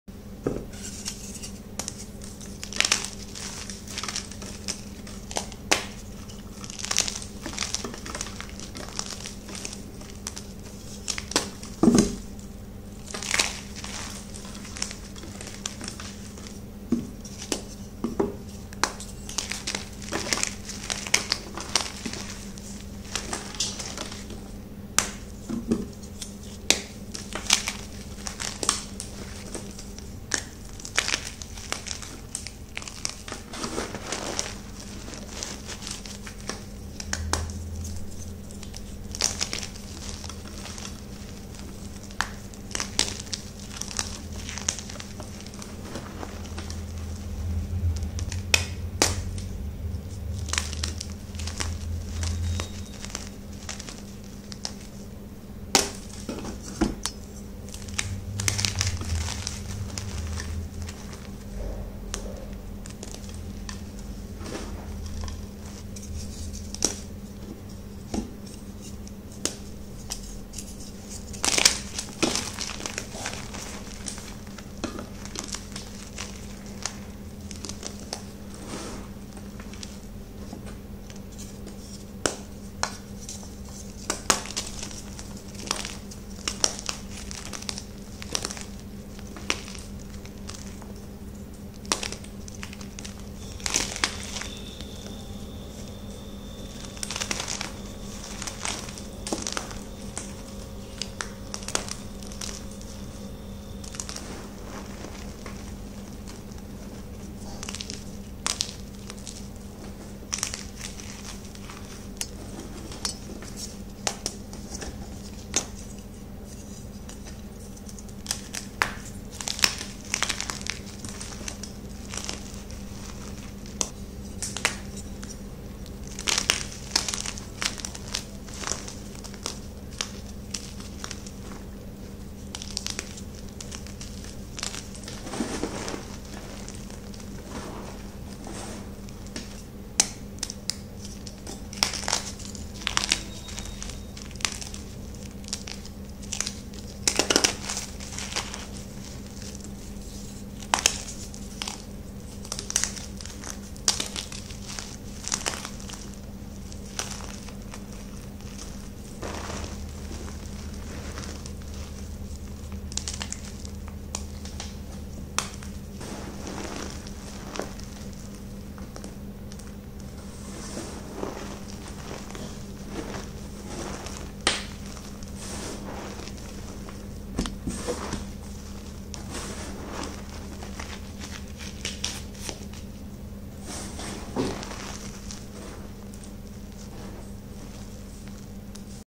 Crispy Crunchy Gritty Gymchalk Reform | (Read Description) ASMR | ODDLYSATISFYING | SLEEPAID
heyoo sweeties!!👋🏼😚

remember those crunchy blocks i did 4-6 days ago? my new powder it was all raw mix nothing added in it so it was hard and crunchy but i have reformed it again with softner in it so it turned into this 🥰
do you like this? please rate the texture from 1-10

i will be doing evaluation of my new powder series soon so let me know what y'all want me to add into it 😊

❥︎I DON'T ALLOW EDITS/REPOSTS ON ANY PLATFORMS ❌ If you steal my videos i will take strict actions.